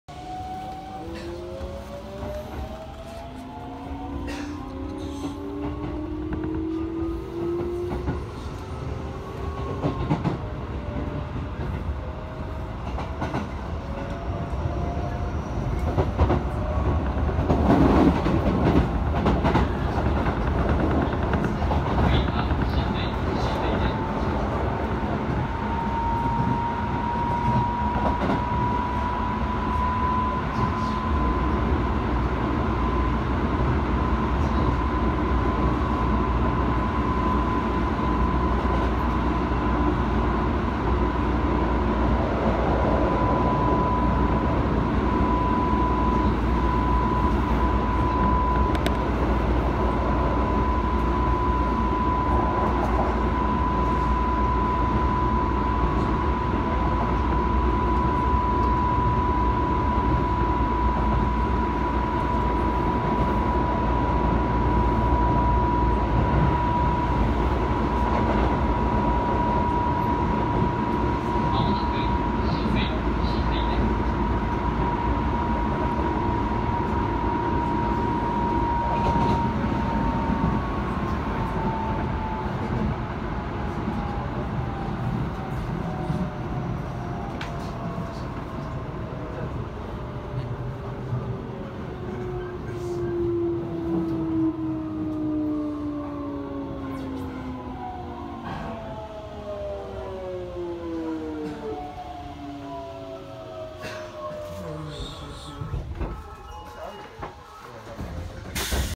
IMG 0357 5宗吾参道至京成酒々井
京成电铁京成本线，宗参吾道站至京成酒酒井站，千叶县印旛郡酒酒井郡
宗吾参道0:13,